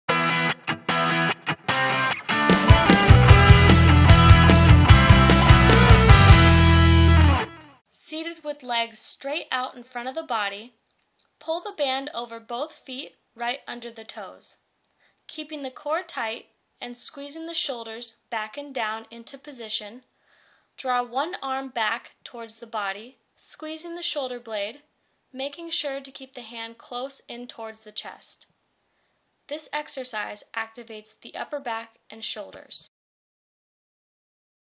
One Arm Seated L Row from 4KOR Fitness
This isometric exercise activates the upper back and shoulders. Ideal for pre, during, and post workout.